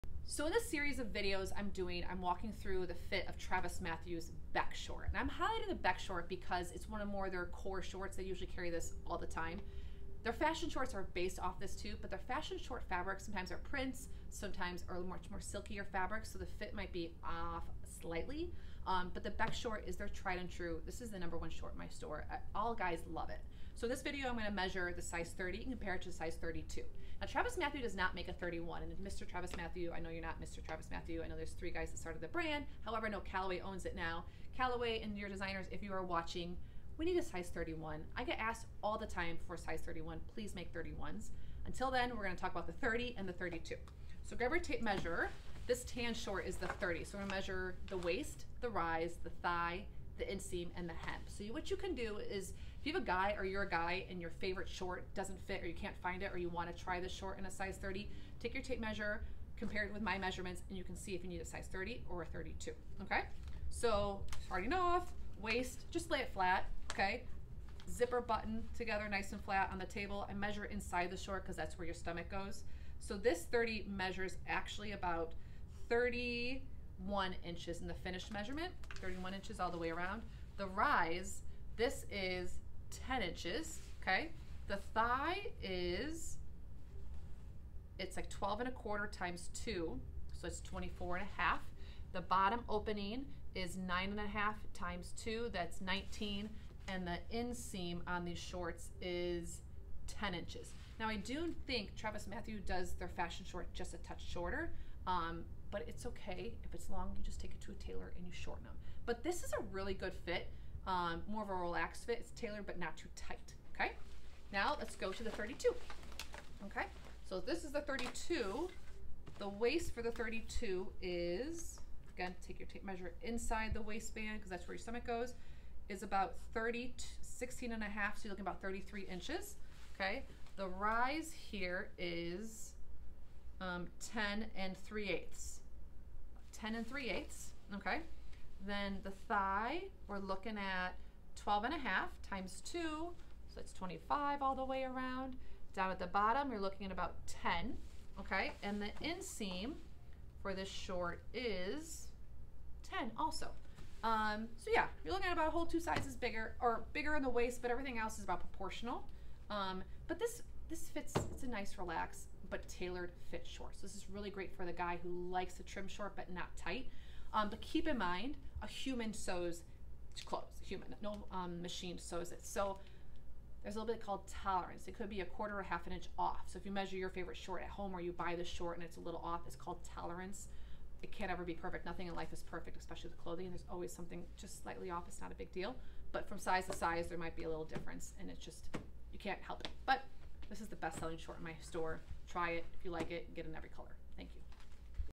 TravisMathew Beck Short Sizing: 30/32
Today I’m going to be walking through the different sizes of the TravisMathew Beck short.  A lot of my clients online will ask me about TravisMathew sizing, and since the Beck short has more consistent sizing than their fashion shorts, I thought I would walk you through one size to the next and teach you how to measure your favorite shorts at home to get the right fit.  I also want to make you aware of tolerance, which is a standard in the clothing industry for about a quarter to a half an inch discrepancy in pieces to take account for human error.  I couldn't recommend these shorts more and I hope you find these videos helpful in finding the right fit for you!

30:
Waist- 31 inches (all the way around)
Rise- 10 inches
Thigh- 12 1/4 x 2 = 24 1/2 inches
Bottom- 9 1/2 x 2 = 19 inches
Inseam- 10 Inches

32:
Waist- 32 inches
Rise- 10 3/8 inches
Thigh- 12 1/2 x 2 = 25 inches
Bottom- 10 x 2 = 20 inches
Inseam- 10 inches